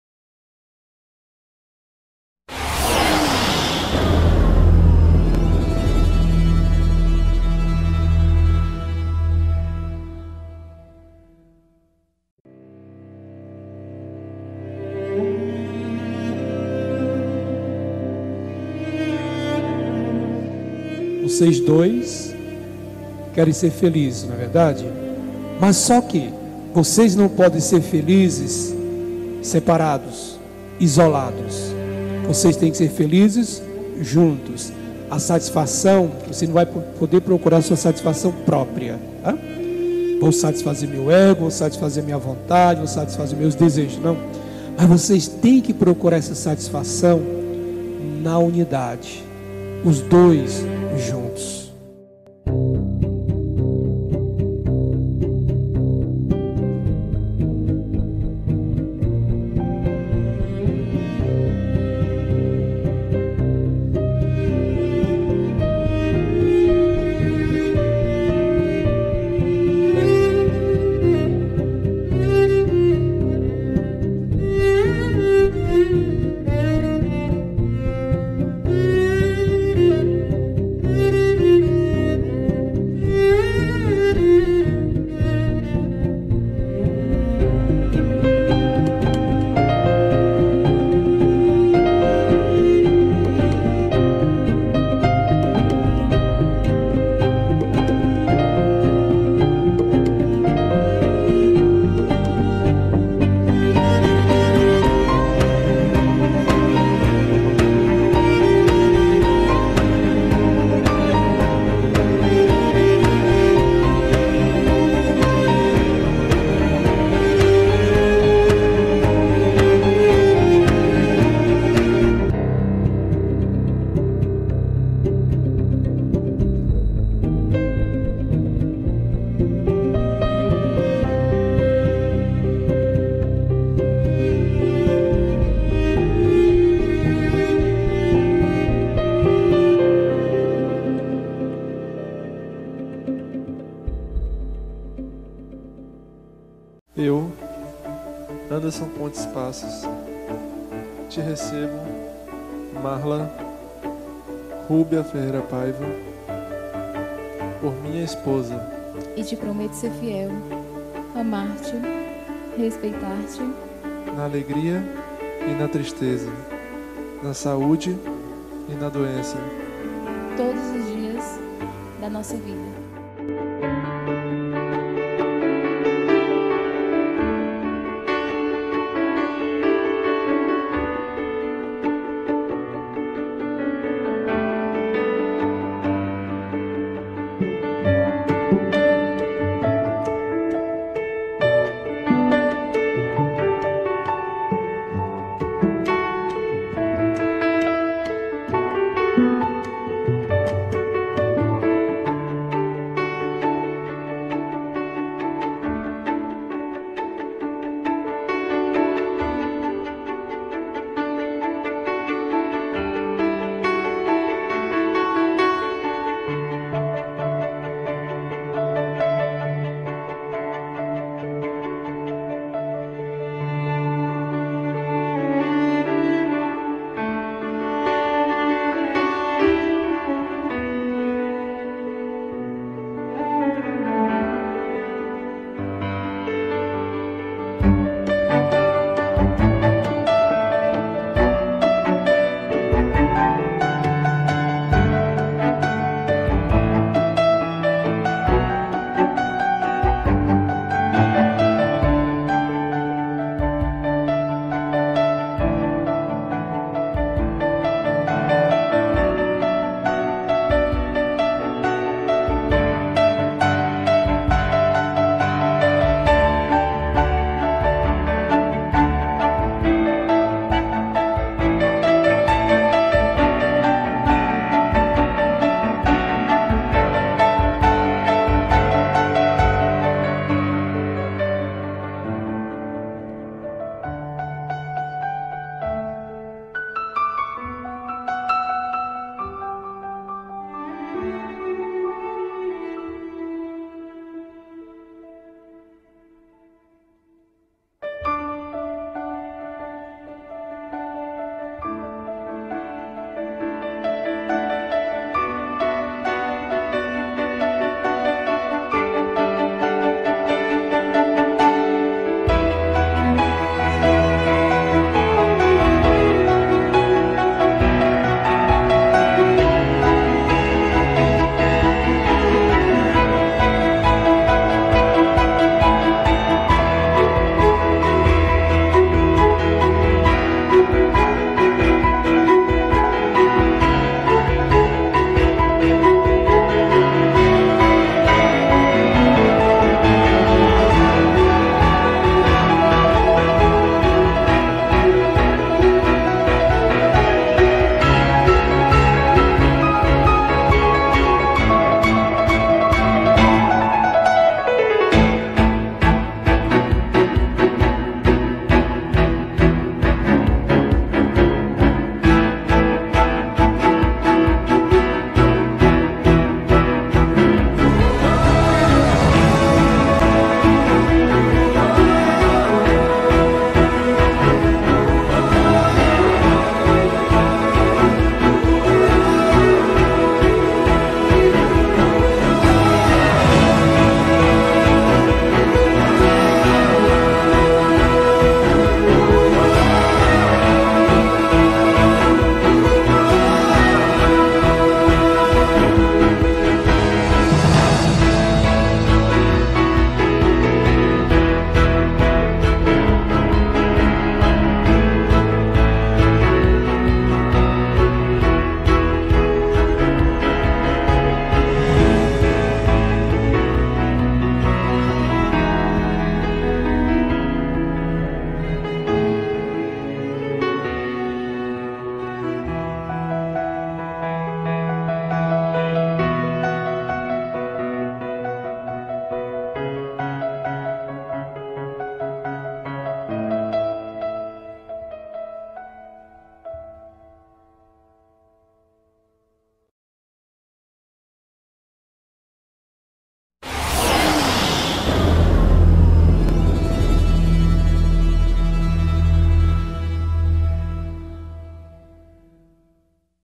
Preview: Casamento Marlla e Anderson
Sobral, 09 de janeiro de 2016.
Estúdio Olhar Digital: (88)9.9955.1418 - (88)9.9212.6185 WhatSap

Celebração: Igreja Nossa Senhora da Conceição (Sé)
Recepção: Buffet Dona Flor